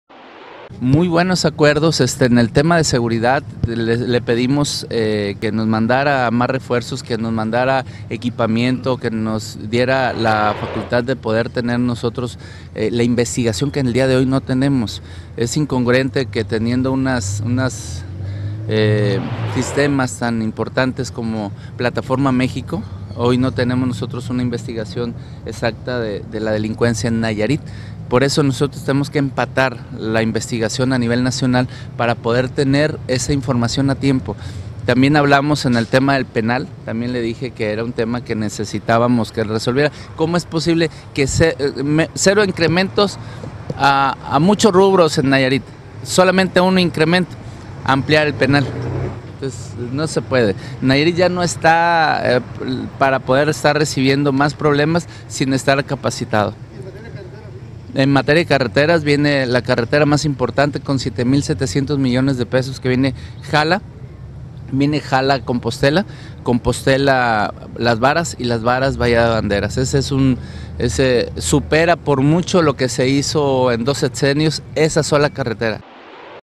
ENCUENTRO CON CALDERON FUE PRODUCTIVO PARA NAYARIT
EN LO QUE RESPECTA AL DIALOGO QUE SOSTUVO CON EL PRESIDENTE FELIPE CALDERON HINOJOSA, SANDOVAL CASTAÑEDA ABUNDO QUE HABLO DEL TEMA DE LA SEGURIDAD PUBLICA, DEL CEFERESO DEL RINCON Y DE INFRAESTRUCTURA CARRETERA...